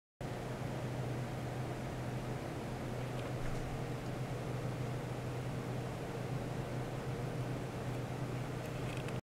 空中を伝搬する超音波の基礎実験（超音波システム研究所）
超音波の基礎実験
－空中超音波の伝搬による表面弾性波の検討－

この動画は、空中超音波の送受信に関する

参考
超音波による音響特性テスト